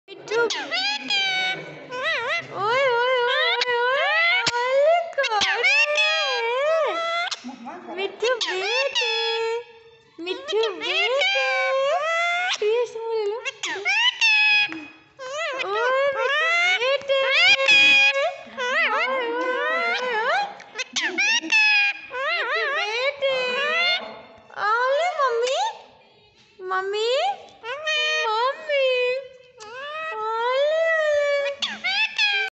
parrot giving me kiss, happy to see me after long time.we miss u mithu 🦜🦜🦜🦜
parrot 🦜 I wish u were with us